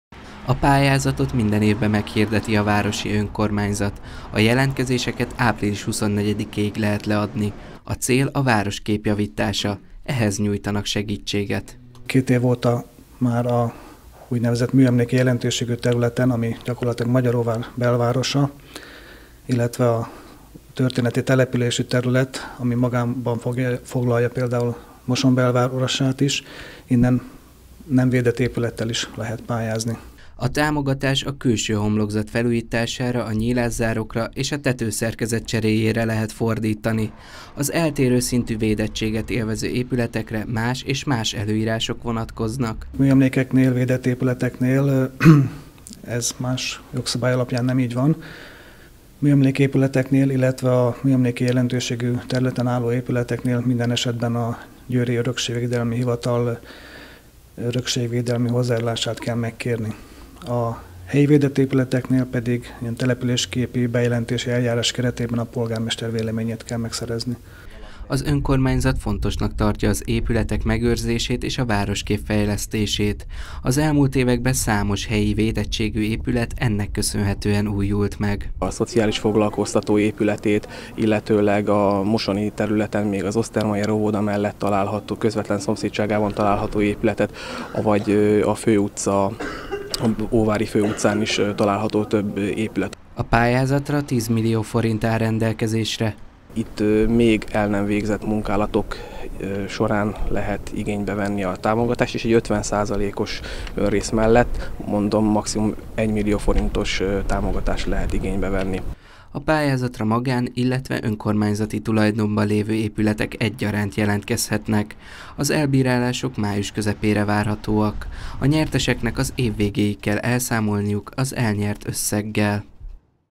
Pályázat épületfelújításra
Mosonmagyaróvár Város Önkormányzata idén is pályázatot hirdet a védett épületek megőrzéséért és illetve a városkép javításáért. A felhívásra április 24-ig lehet jelentkezni. Az összeget a külső homlokzat felújítására lehet fordítani. Ezzel segítve az utcakép szebbé tételét.